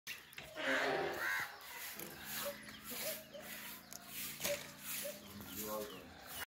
Off season custard apple harvesting
This sithaphal fruit is harvested date is 1st april 2020 and still there is flowering and new fruits are set on the plant